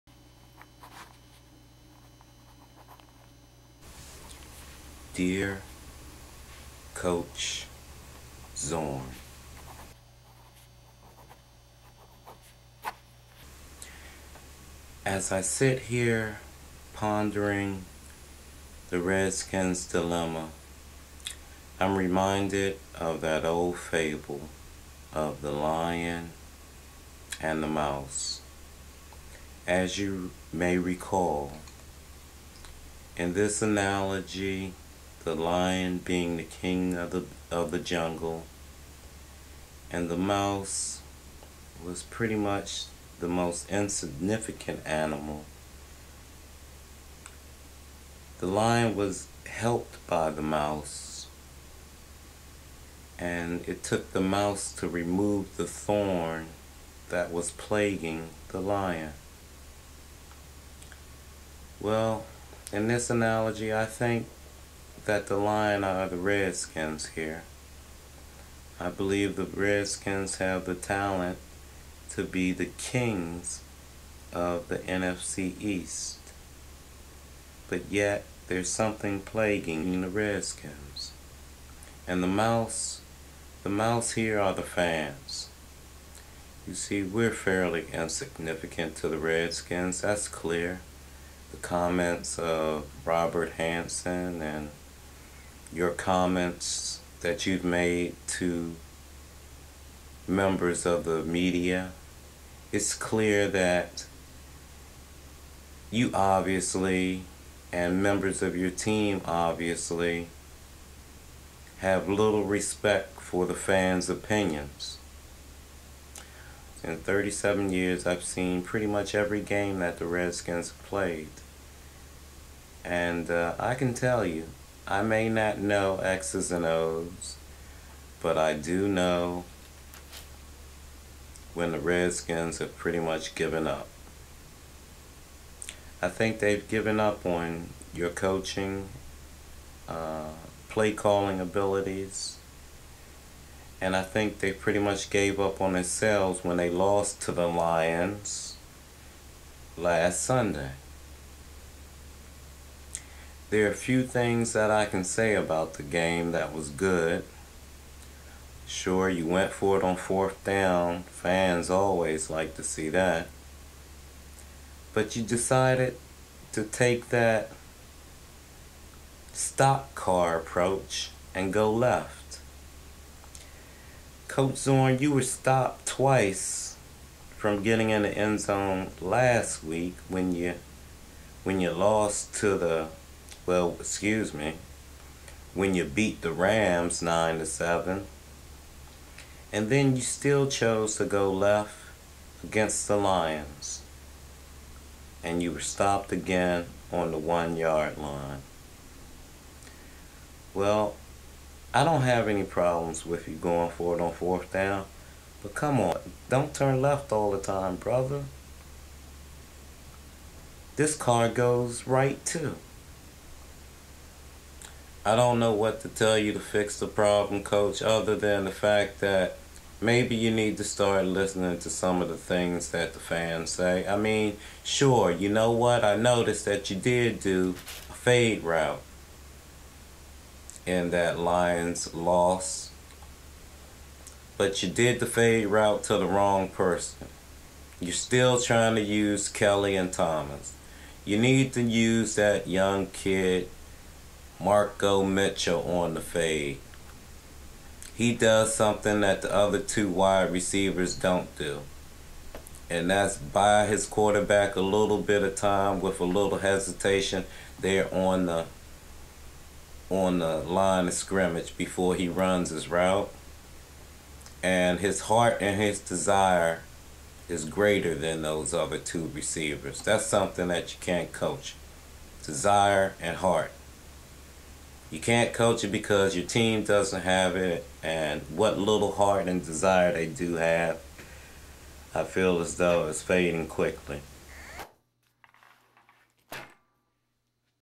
Dear JIM ZORN, Please Remove the Thorn
Letter to Washington Redskins Coach Jim Zorn after lost to Detroit Lions. PART 2